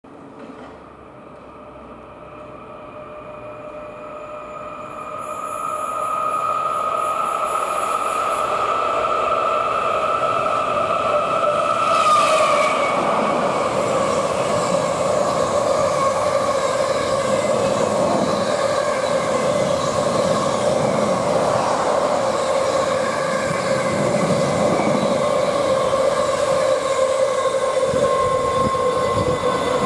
86639/86607 with 86613 dit 4m11 Coatbridge - Crewe liner, 20th March 2018
ON A MISSION !! Screaming through Lancaster.